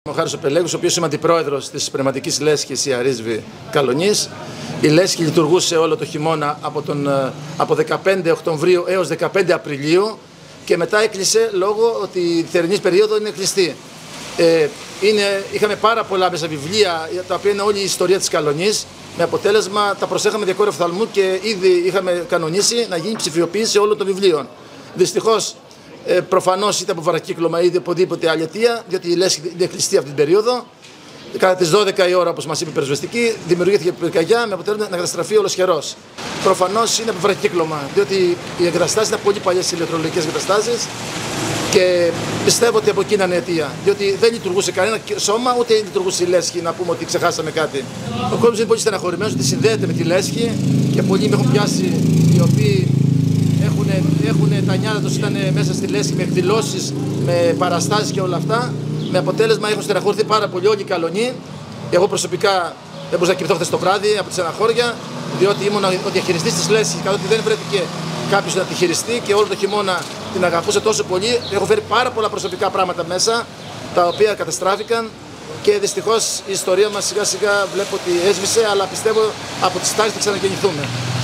Στις στάχτες η πολιτιστική ιστορία της Καλλονής
Ολοσχερώς το κτήριο του Πνευματικού Κέντρου της Λέσχης Αρίσβης τη νύχτα της Τετάρτης 15 Μαΐου. Κάτοικοι λένε στο «Ν» ότι έμειναν «μόνο τα ντουβάρια»